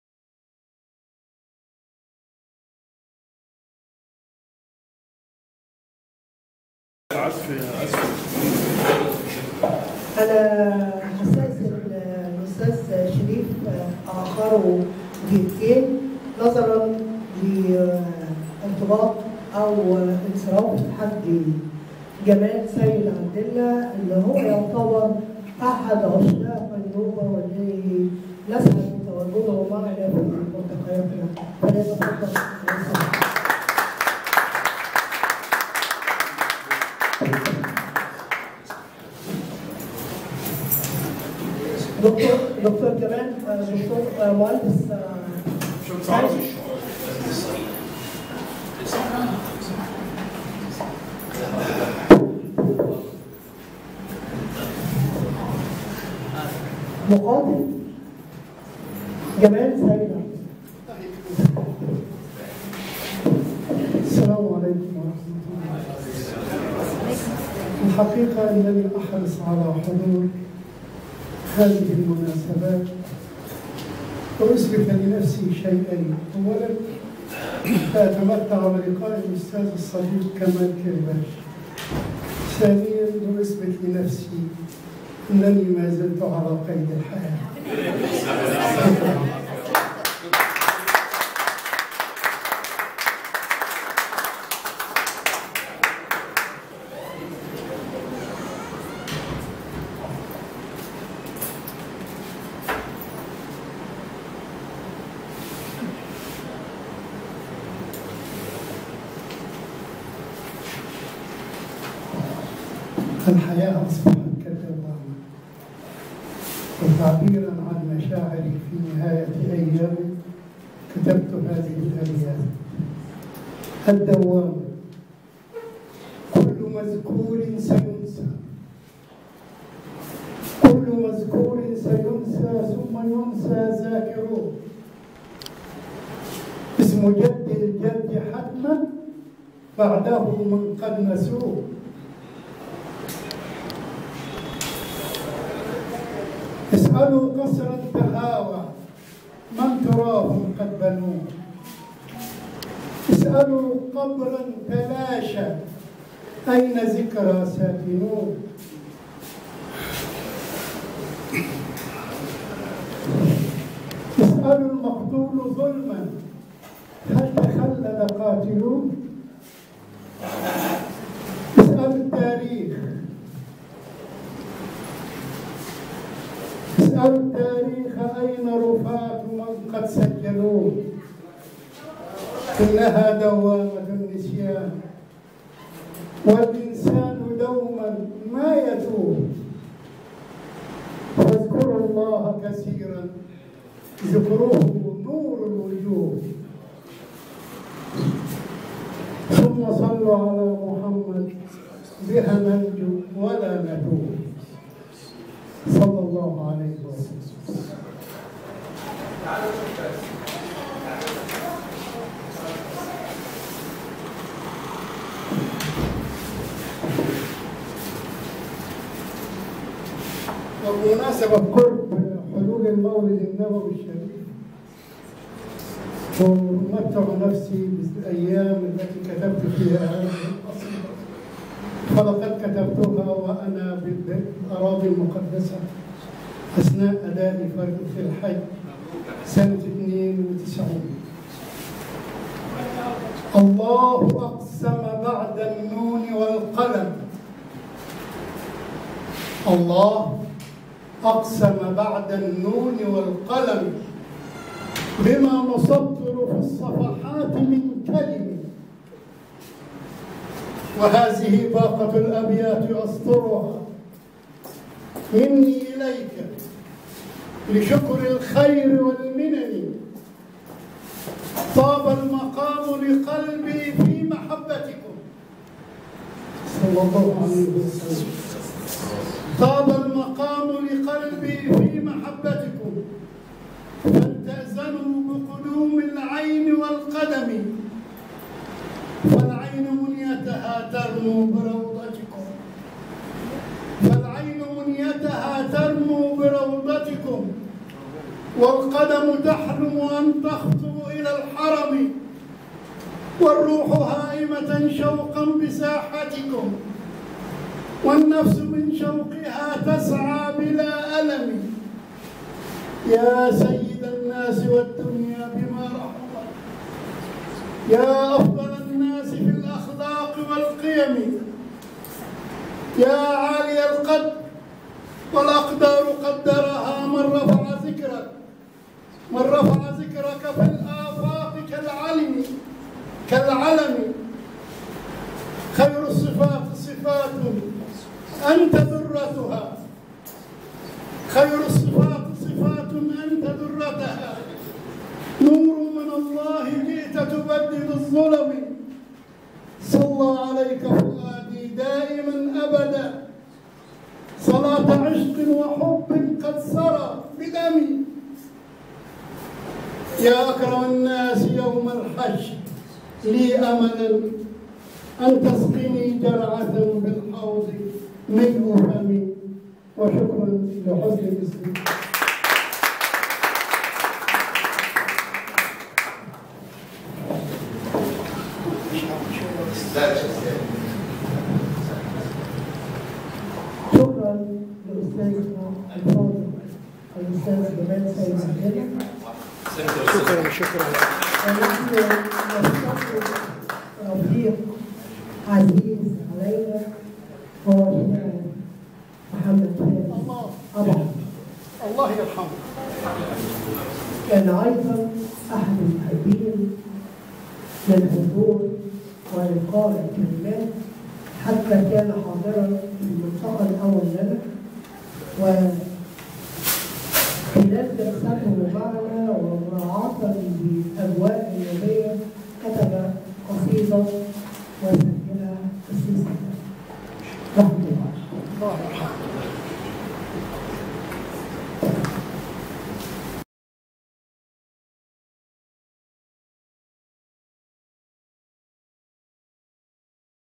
منتدى شعراء النوبة/مهرجان النوبة/اليوم الثاني/الشاعر/جمال سيد/السبت24/9/2022/ مؤسسة الحسيني الثقافية
أمسية ثقافية
منتدى شعراء النوبة
مؤسس ورئيس مجلس إدارة المنتدى
الشاعر / كمال كرباش
مهرجان دعوة حب لعشق النوبة / اليوم الثاني
الشاعر المحب للنوبه الأستاذ/جمال سيد عبد اللاه
المكان : مؤسسة الحسيني الثقافية
شارع عين شمس رقم 45 / حلمية الزيتون
عنيبة الخير / أسوان / القاهر/ مصر